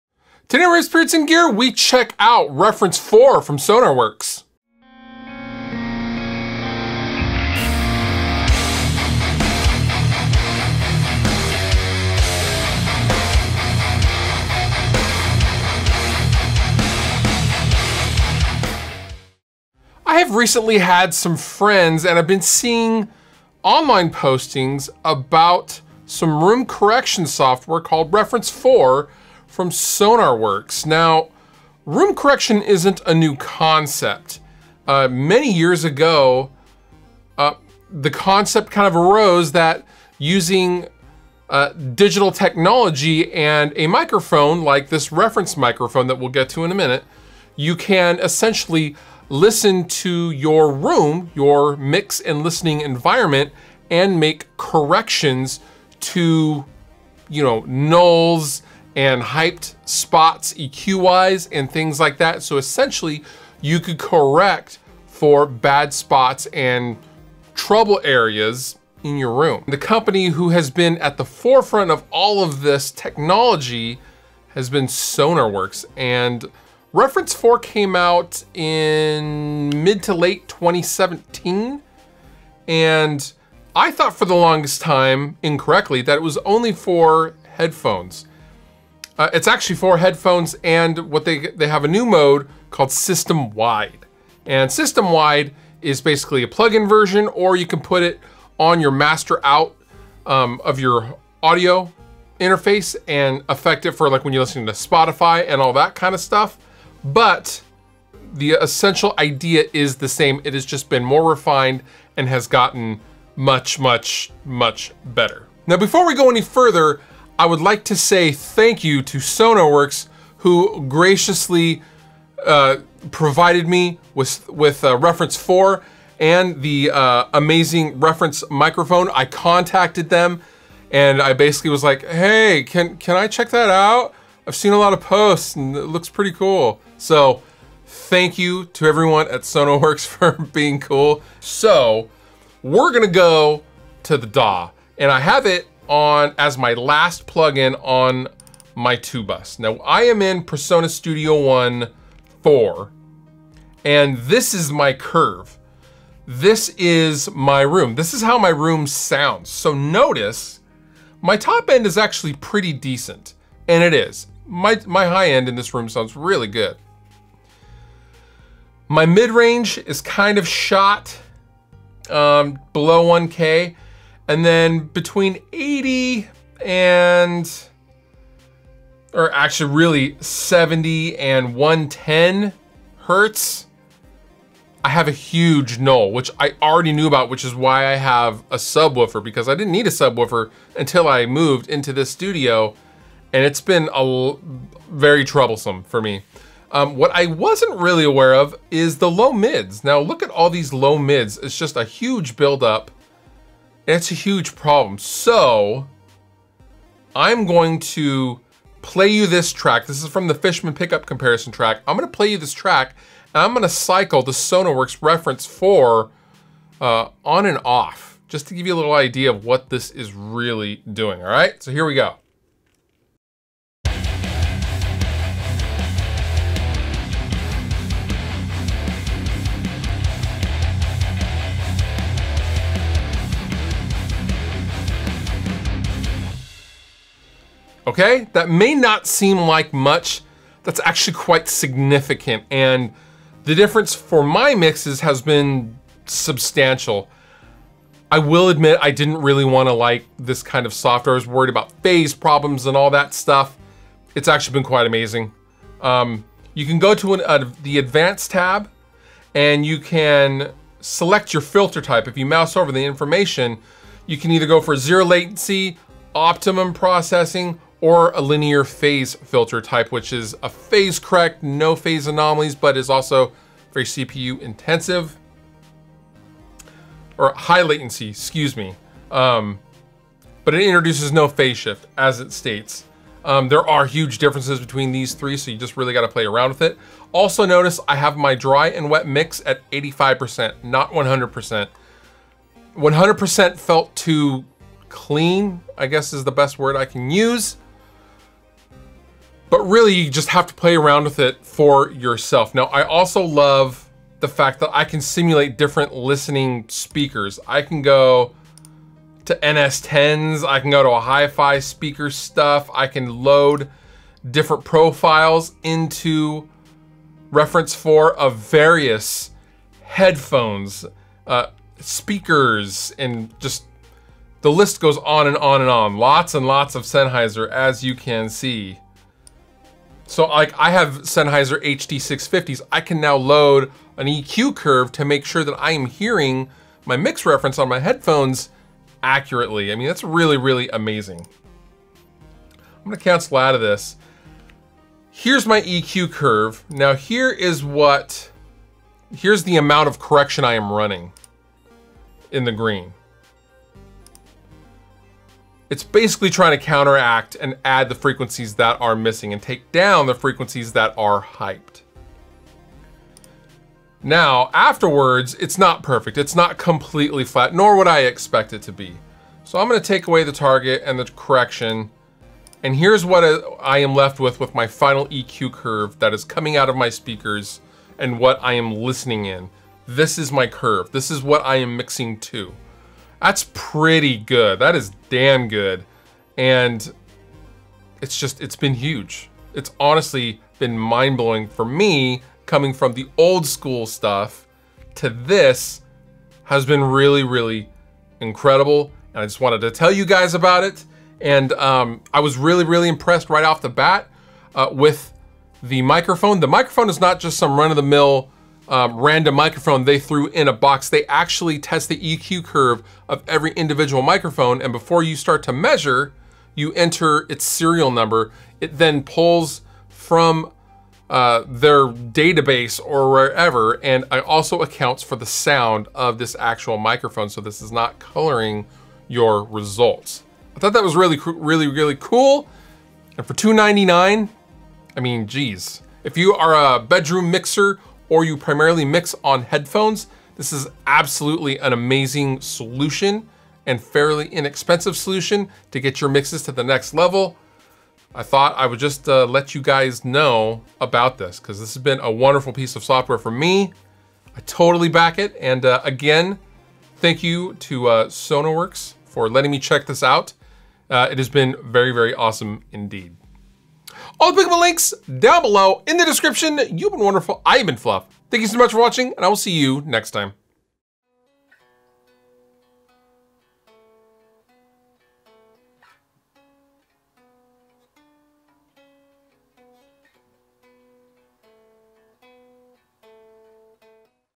Digital Room Correction?! Sonarworks Reference 4!
Can digital room correction save your mix? Lets find out!

The Gear:
This Camera Body
Or This Camera
With This Lens
With This Tripod
With This Shotgun Mic